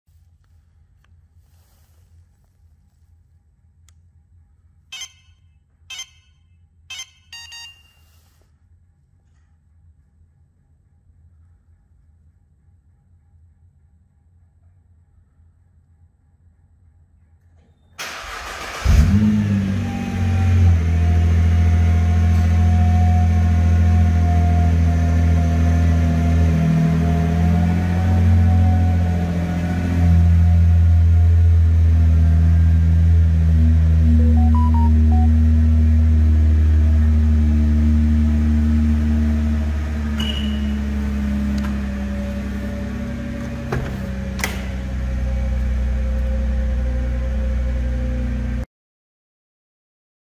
Nissan GTR Engine Remote Start
Engine Remote Start on this Nissan GTR 35. Lights illuminate beautifully on cold mornings.